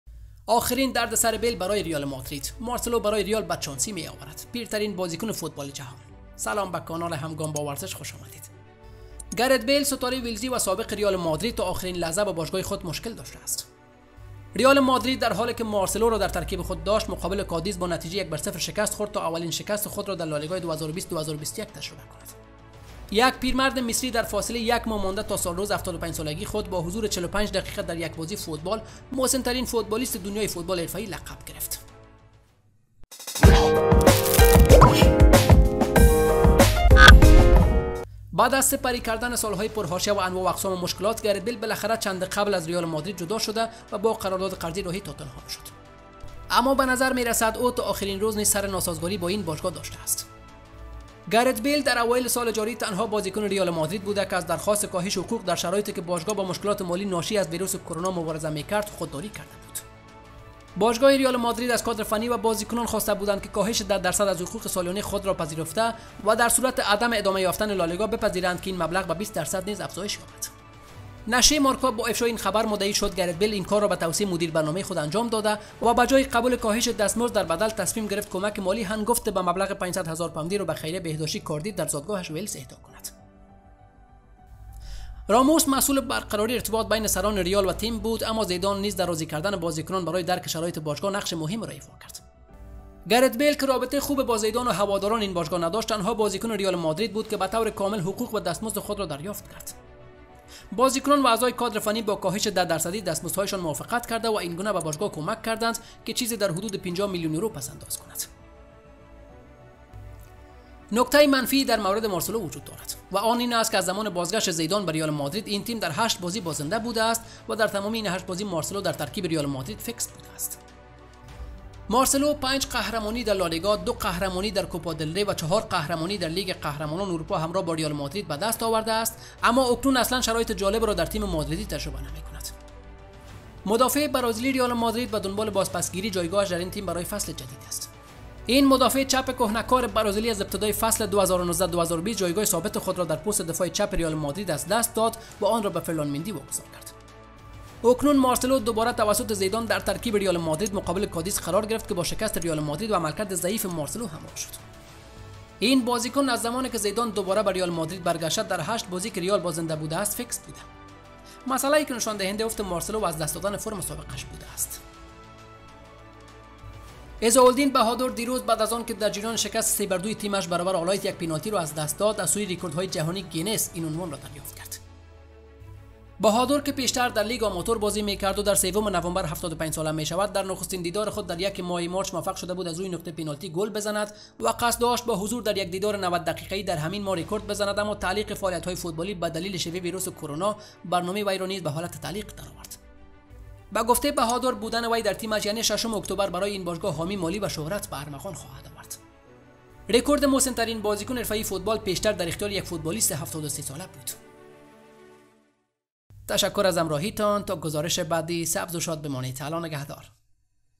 آخرین دردسر بیل برای رئال / مارسلو برای رئال بدشانسی می‌آورد / پیرترین بازیکن فوتبال جهان
سلام و وقت بخیر:- گرت بیل، ستاره ولزی و سابق رئال مادرید تا اخرین لحظه با باشگاه خود مشکل داشته است. رئال مادرید در حالی که مارسلو را در ترکیب خود داشت، مقابل کادیز با نتیجه 1 بر 0 شکست خورد تا اولین شکست خود را در لالیگا 2020/21 تجربه کند. یک پیرمرد مصری در فاصله یک ماه مانده تا سالروز 75 سالگی خود، با حضوری 45 دقیقه در یک بازی فوتبال، مسن‌ترین فوتبالیست دنیای فوتبال حرفه‌ای لقب گرفت.